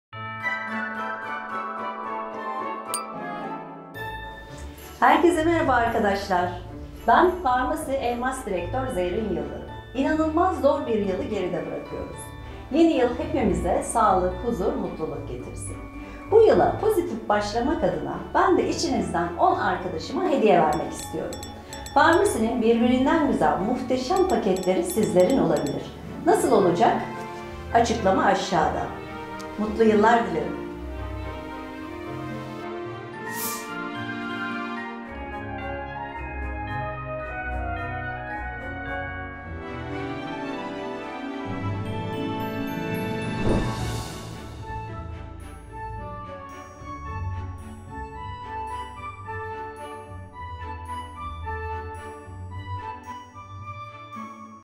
ZERRİN YILDIRIM YILBAŞI ÇEKİLİŞİ 🎀🎁| #2021
Yeni yıla pozitif girin istedik ve sizler için birbirinden güzel yılbaşı hediyeleri hazırladık 🎀🎁
YORUM YAPMAYI UNUTMA 🎀
Çekilişle belirleyeceğimiz 10 kişiye birbirinde güzel 10 Farmasi Seti hediye;

► 1 kişiye DR.C.TUNA Pure  Rose  Set
► 1 kişiye White Correct Aydınlatıcı Set
► 1 kişiye DR. C. TUNA TEA TREE 5’li Bakım Seti
► 1 kişiye Farmasi Başlangıç Seti
► 1 kişiye Shild Men Erkek Set
► 1 kişiye Winter L’amour Body Set
► 1 kişiye DR. C. TUNA Aynısefa Bakım Seti
► 1 kişiye Farmasi Makyaj Başlangıç Seti

 Toplamda 10 adet Set hediye 🎁🐞

⬇️YAPMAN GEREKEN ⬇️

4- Bu gönderinin altına yorum yapmak

⌛️ Çekiliş’e son katılım tarihi 31 Aralık 2020 saat 23.59’dur. Kazananlar yine bu sayfadan 3 Ocak 2021 Tarihinde Açıklanacaktır. ⌛️

Şimdiden herkese bol şans 🎀🎁🐞